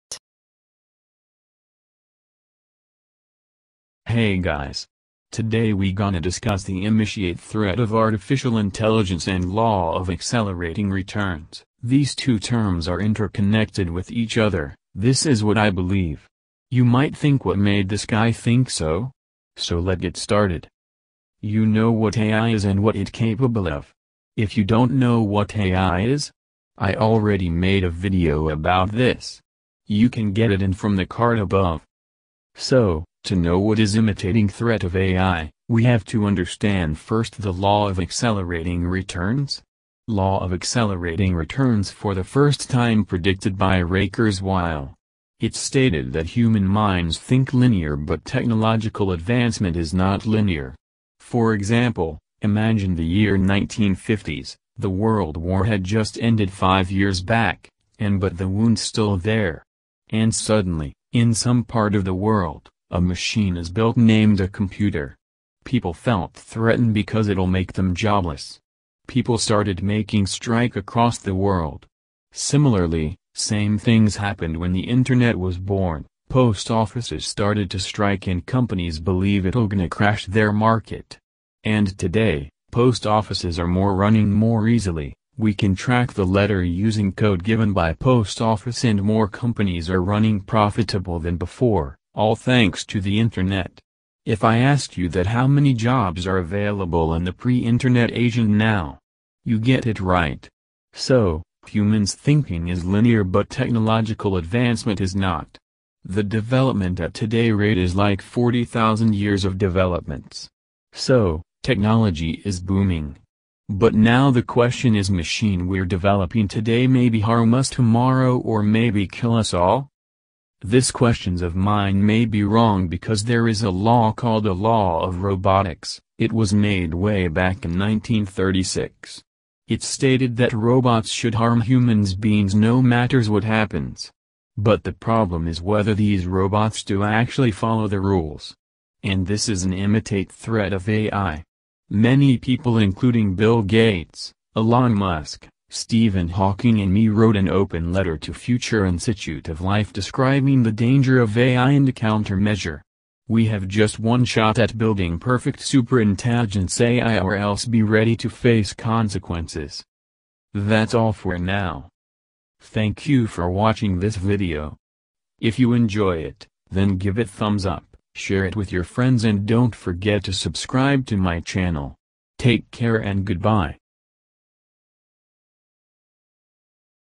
Rise of Artificial Intelligence
Artificial Intelligence is booming but the problem is if it's cross the limit where people don't understand what machine is doing, then what'll happen next?